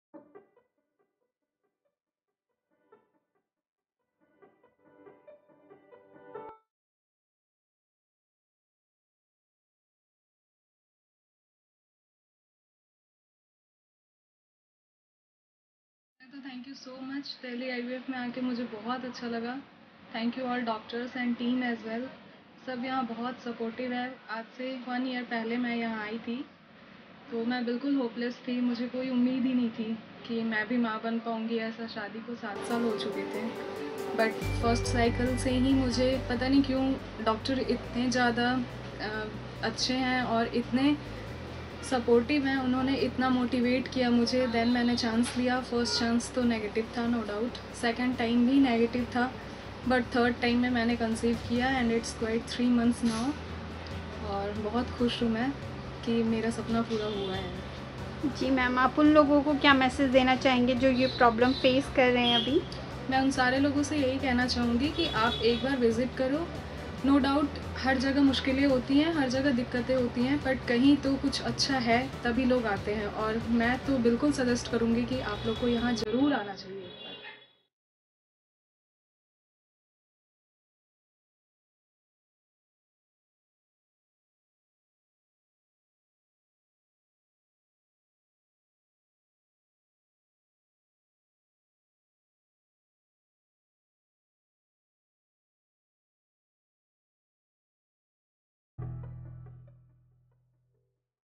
IVF Happy Story Of Mrs. Neha Conceiving After 7 Years Of Marriage|Best IVF clinic India
This story is of Mrs. Neha who has been married for almost 7 years but unfortunately couldn’t become mother because of infertility, when she came to us for treatment she was completely hope less, her first IVF cycle and second IVF cycle were both unsuccessful but still she went for the third IVF cycle which came out as successful. Now it’s been 3 months of her pregnancy and she is happier than ever that her dream came true. She suggests couple with infertility problem to come and visit Delhi IVF for positive results.
📍Address: 23, Todar Mal Lane, Bengali Market (Opp. Nathu's Sweets), Near Connaught Place, New Delhi.